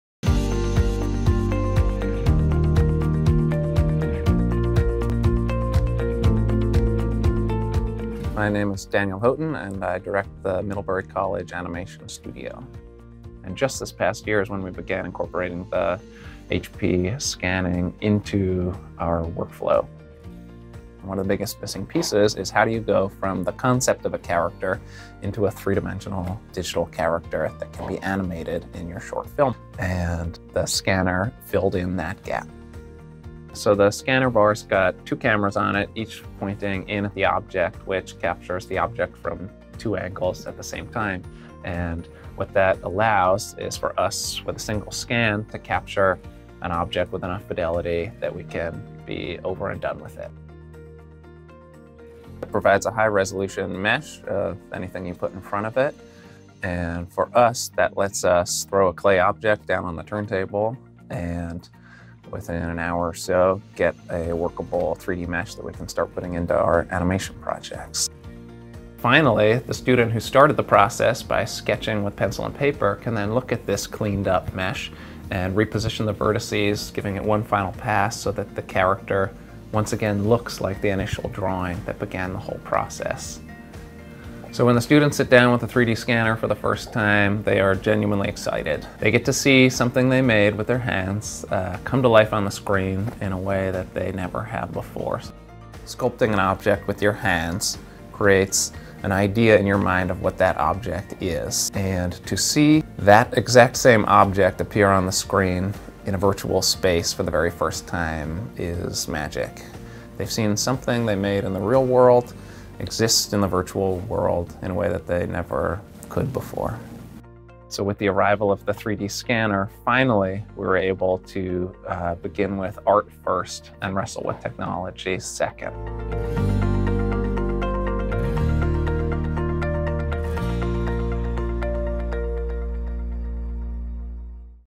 HP 3D Structured Light Scanner Pro S3 Case Study - Digital Animation
Digital Animation Studio at Middlebury College chooses
HP 3D Structured Light Scanner Pro S3 with Dual Camera to enhance creativity.

Incorporating the HP 3D Scanner into the digital animation workflow enables creators to focus on "art first and technology second."